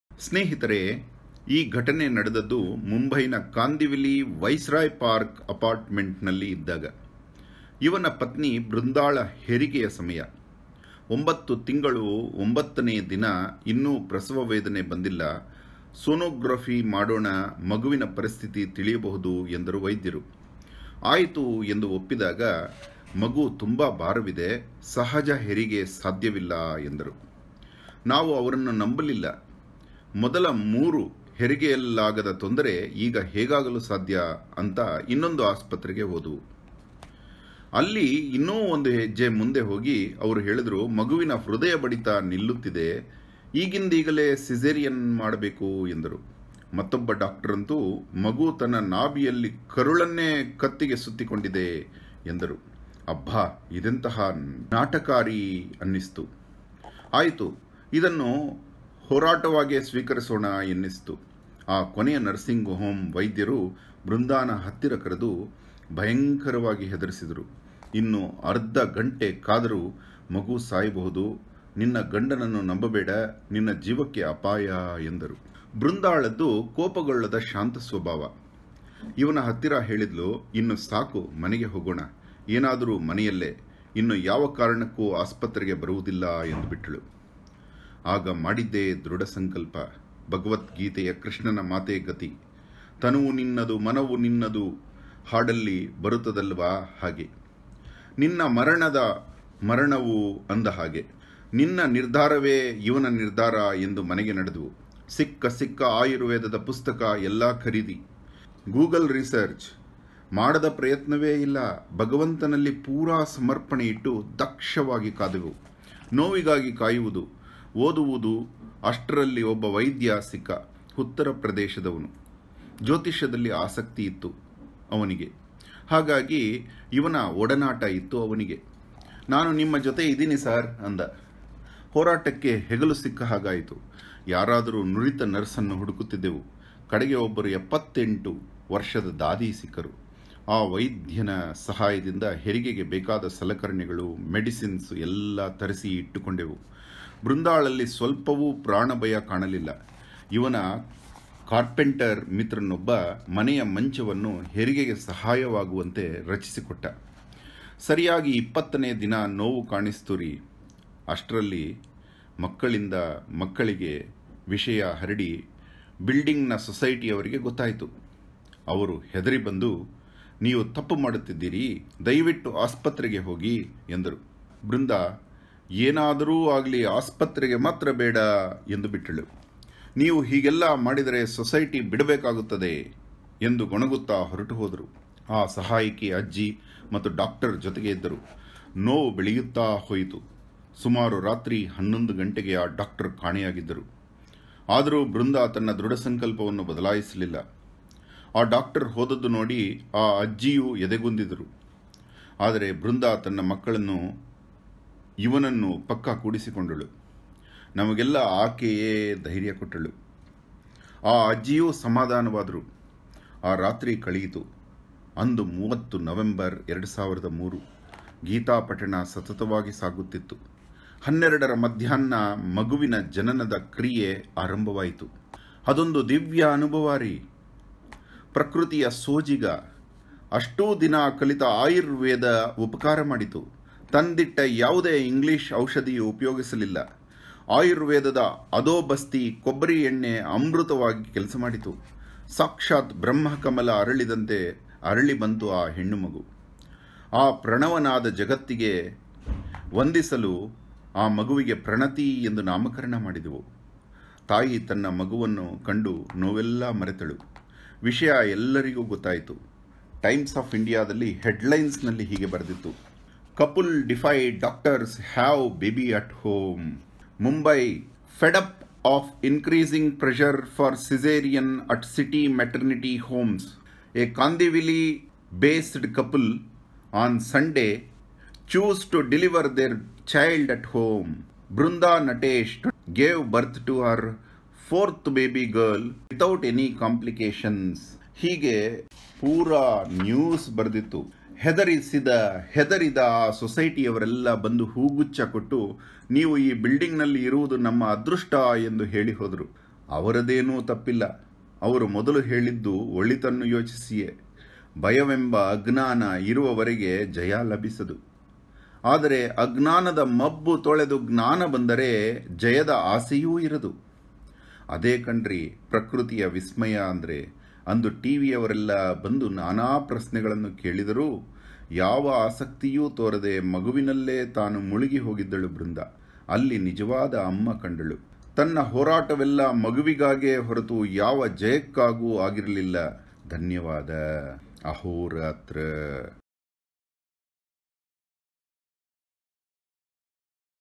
Couple defy doctors, have baby at home
TNN | Dec 6, 2003, 02:41 IST
MUMBAI: Fed up of the increasing pressure for Caesareans at city maternity homes, a Kandivali-based couple, on Sunday, chose to deliver their child at home.
Vrinda Natesh, 28, gave birth to her fourth baby girl with the help of a retired nurse from Kerala without any complications.
Vrinda and her husband Natesh, who live in Thakur village, Kandivli, said they were being pressurised by gynaecologists to undergo a Caesarean just two days after her due date of November 21.
Natesh, an astrologer, told TNN that he was very suspicious of the doctors, as they seemed too eager to go for a Caesarean operation. They claimed the baby was weighing 4.7 kg and that she had become too heavy to be delivered normally, he said.
An obstetrician who charges Rs 6,000 for a normal delivery can slap a fee of Rs 20,000 for a Caesarean. Defying the doctorsâ€™ orders, the couple made arrangements to have their baby at home. Eleven days past the due date, Vrinda delivered the baby weighing 4 kg.
We did not induce labour but chose to wait, Natesh said. While two of her children were born normally, one was delivered by C-section. During the second delivery in 1996 we were forced to undergo an operation as the doctor claimed the baby was in a breach position (legs first),said Natesh.
For the fourth delivery, the couple did not even have a doctor around but allowed their children to witness the process. The baby came out smoothly after 12 hours of labour at 12.15 p.m. on Sunday. Though doctors had suggested Natesh to keep an ambulance as a standby for an emergency, he refused.
However, Natesh was prepared to take his wife to Nanavati hospital in case of complications. An elated Natesh feels other young couples should not follow doctors blindly. We should question them. In the United States, Caesarean is undertaken as a last option but here it is done at the drop of a hat,he noted.